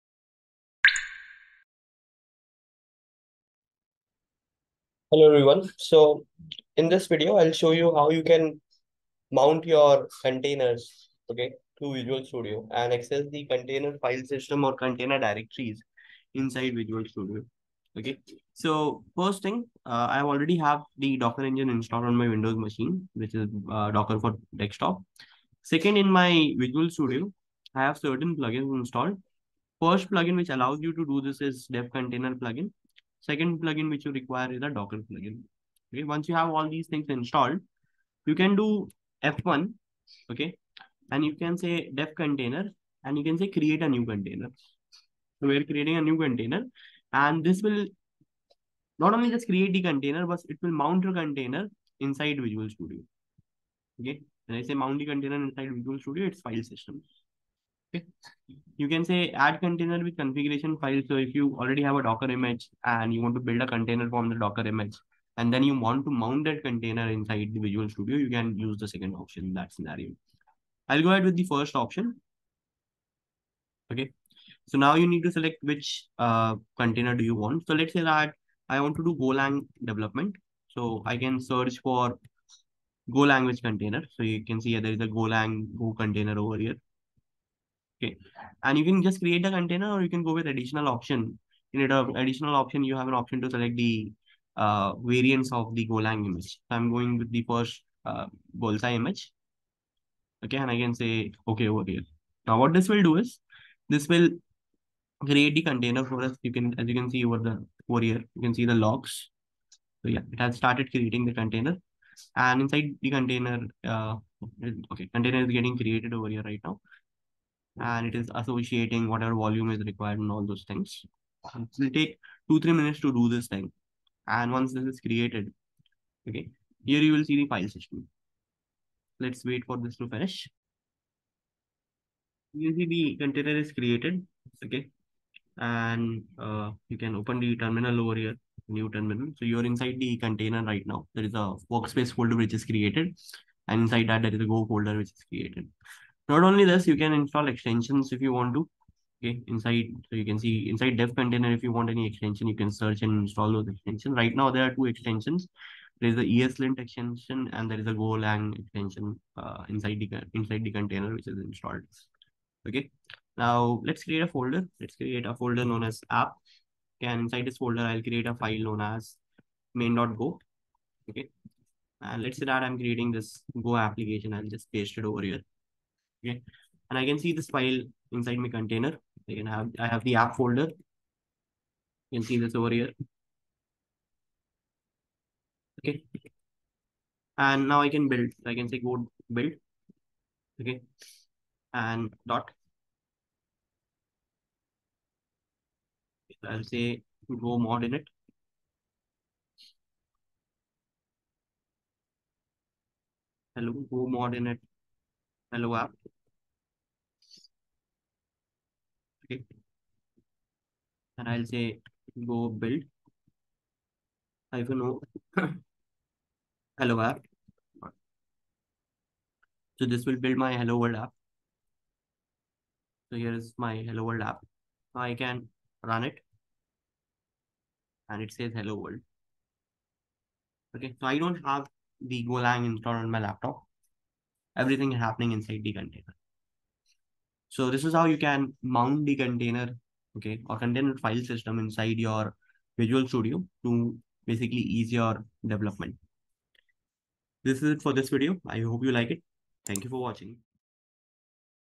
How to Mount Docker Container Instance In Visual Studio Code
In this step-by-step tutorial, we'll show you how to easily mount a Docker container instance from within Visual Studio Code, making it convenient for development, debugging, and testing of your containerized applications.